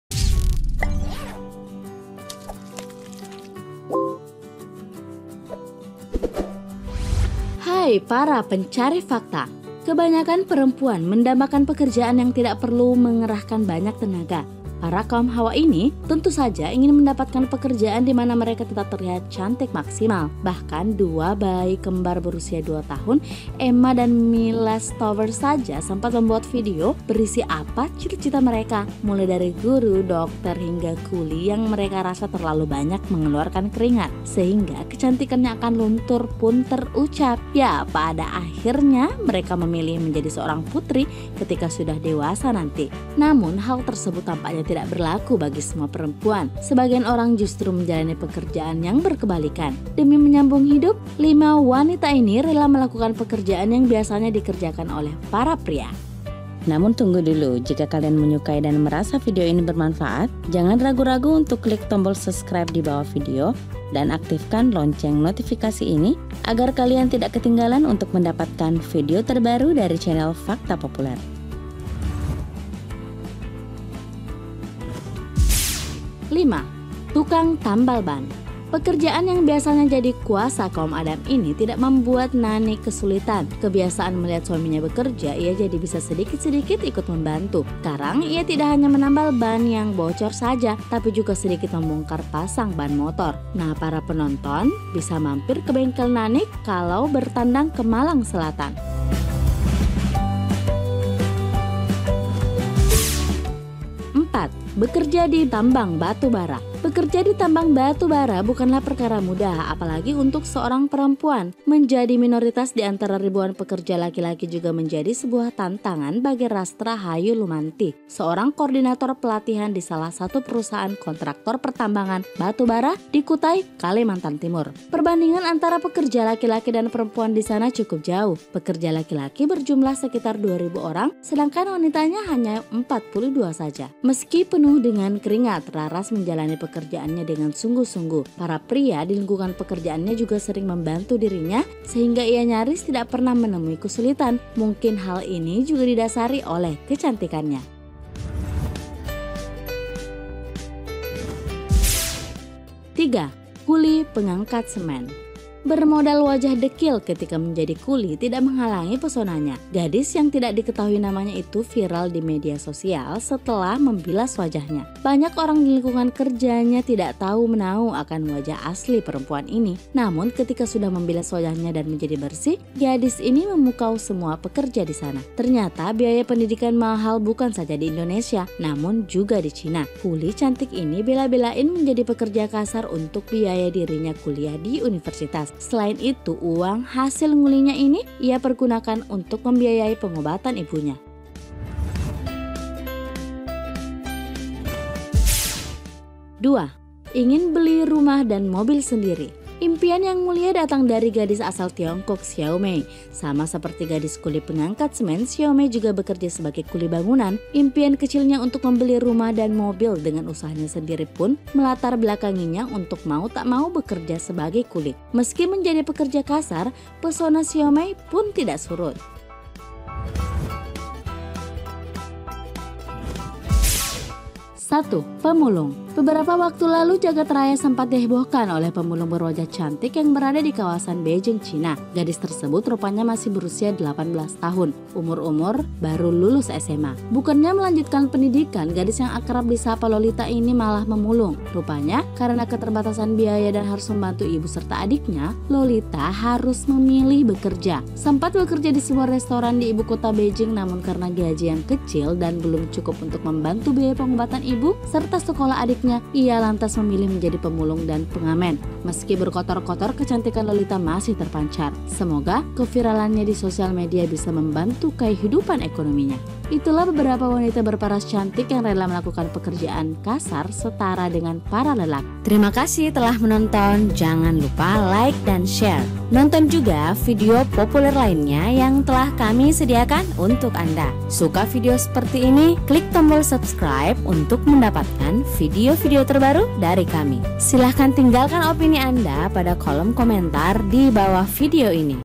5 Wanita Cantik Tak Minder Sebagai Pekerja Kasar
Seperti apakah sosok wanita yang memiliki keinginan tinggi dengan profesi sebagai pekerja keras? Berikut Ulasannya